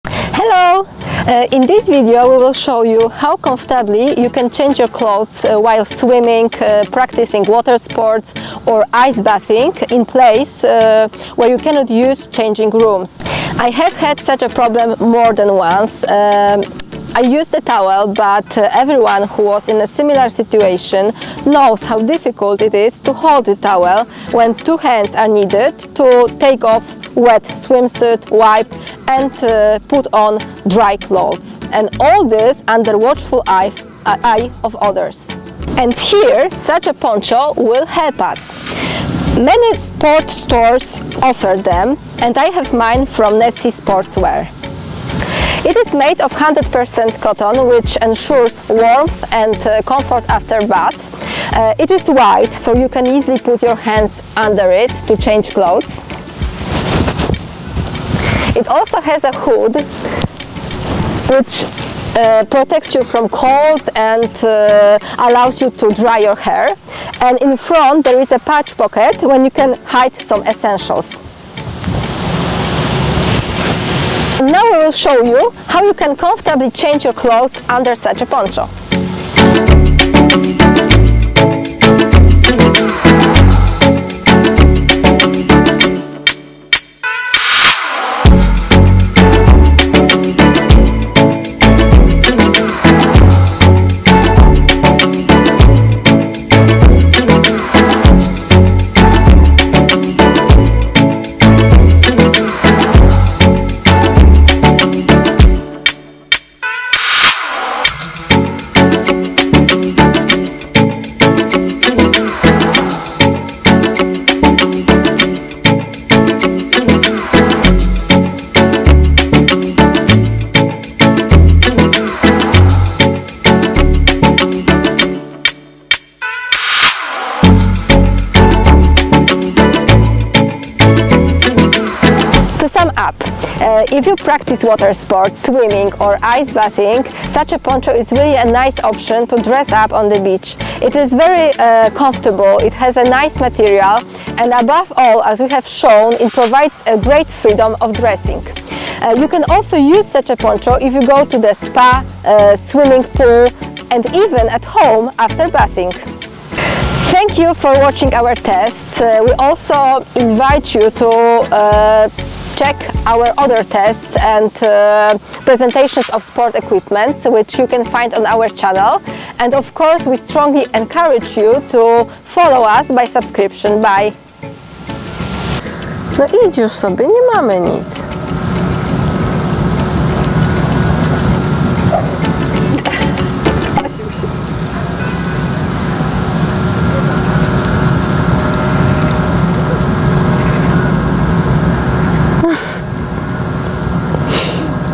Poncho Nessi Sportswear - a convenient way to change clothes on the beach
In this video we are presenting a very cool and useful product from Nessi Sportswear! Their Poncho is a great idea for all those who like to spend their time on the beach, love water sports or ice bathing!